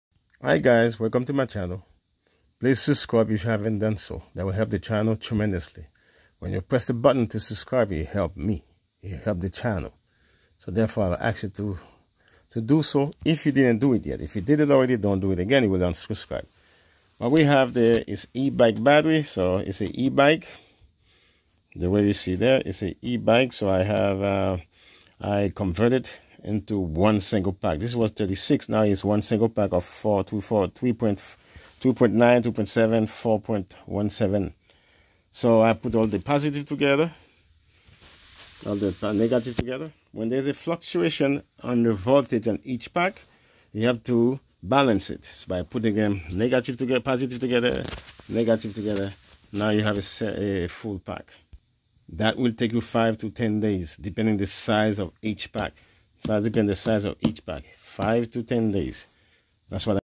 HOW TO BALANCE Lithium Iron Batteries and Cells. The quickest way to place the cells in parallel
The absolute best way to balance cells is connect cells in parallel that are at 80 % SOC or less, and then use a power supply (3.6 V for Phosphate cells, 4.2 V for Lithium Iron)